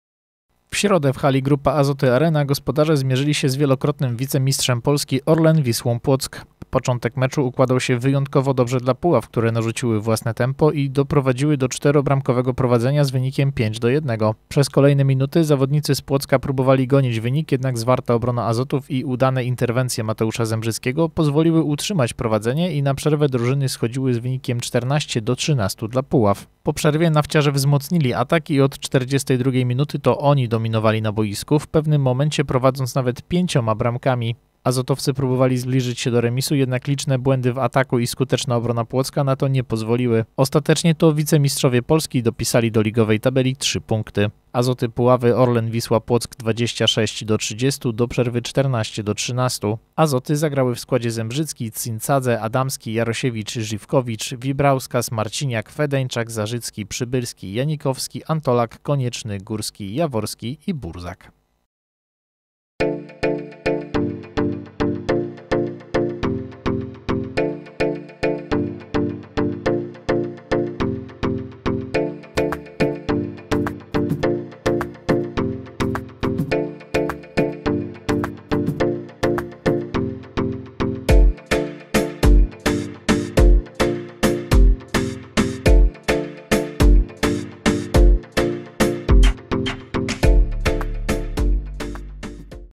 07 09 2023 Nafciarze zbyt mocni dla Azotowców
Początek meczu układał się wyjątkowo dobrze dla Puław, które narzuciły własne tempo i doprowadziły do czterobramkowego prowadzenia z wynikiem 5:1.